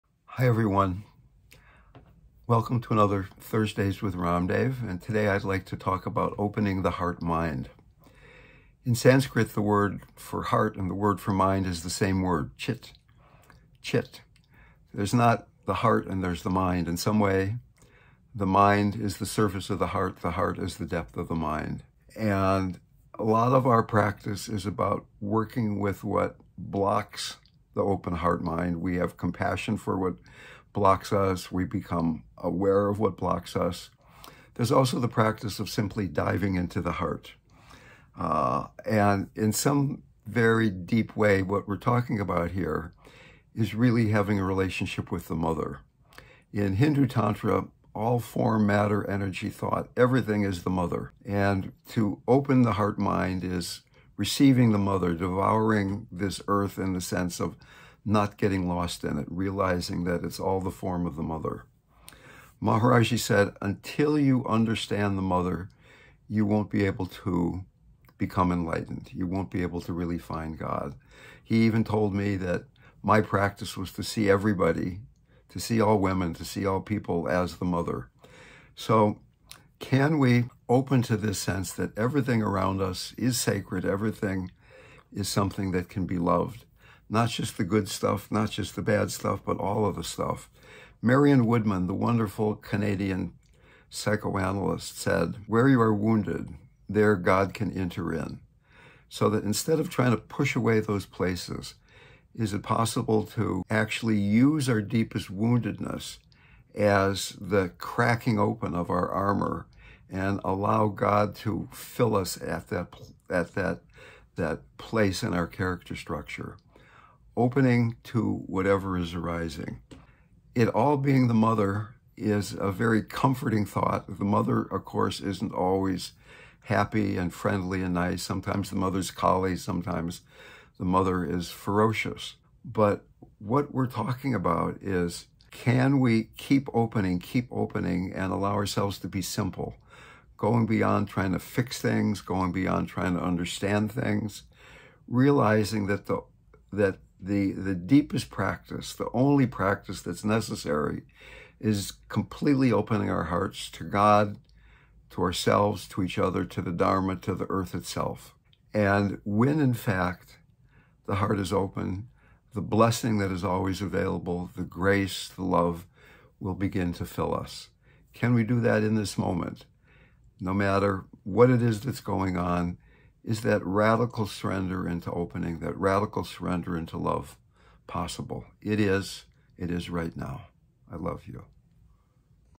Chit: Opening the Heart-Mind
Consider investigating the possibility that all is a manifestation of the divine Mother, divine Consciousness. That all is sacred. That even in our wounding or fright, Love can pour in from our surrendering, our radical opening.

"In some way, the mind is the service of the heart, the heart is the depth of the mind." ⁣⁣
- RamDev⁣ Dale Borglum⁣ ⁣ ⁣⁣⁣ ⁣